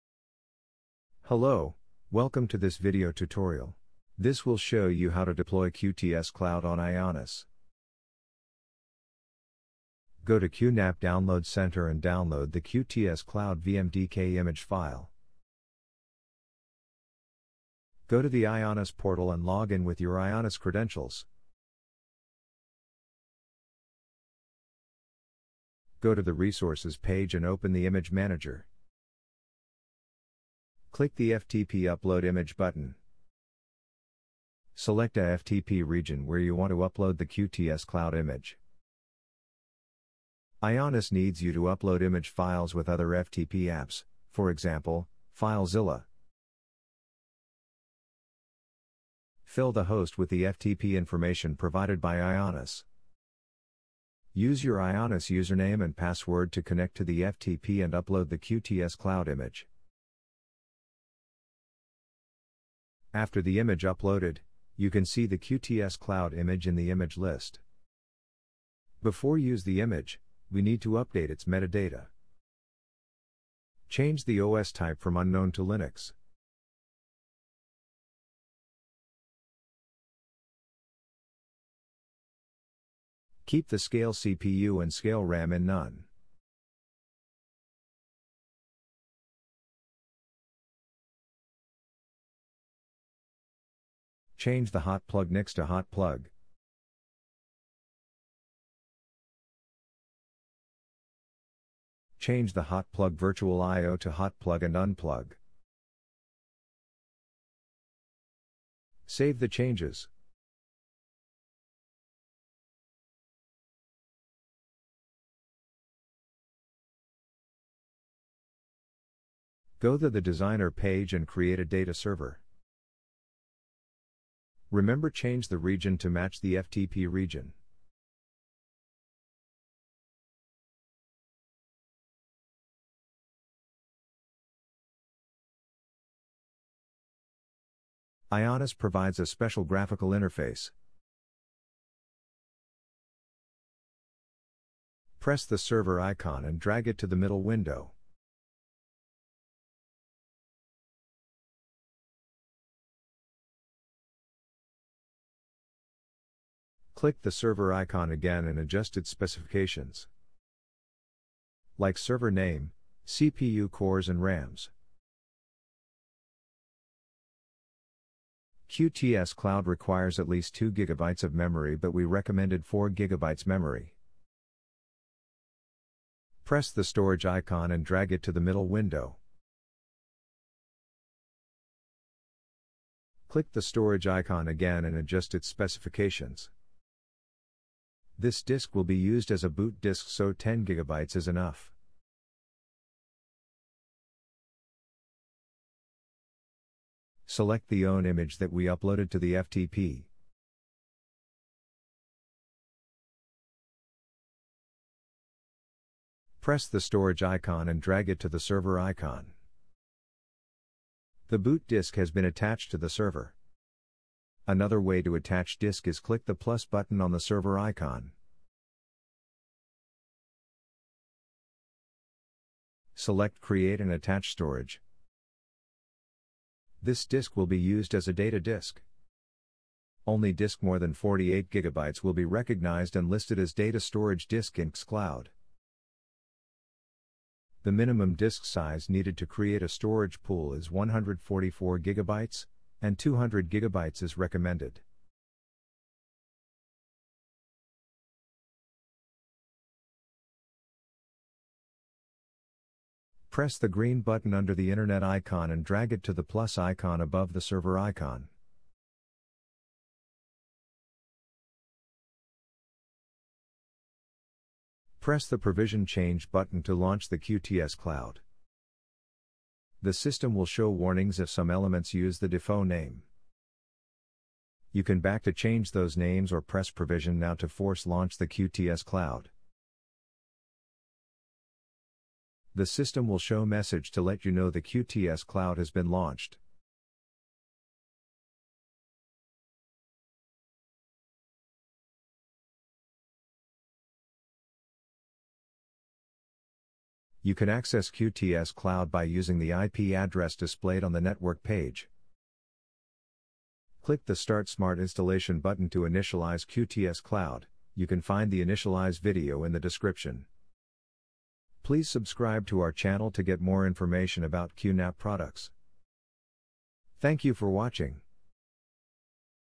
QuTScloud Tutorial | Deploy QuTScloud on IONOS
This tutorial shows you how to deploy QuTScloud on IONOS.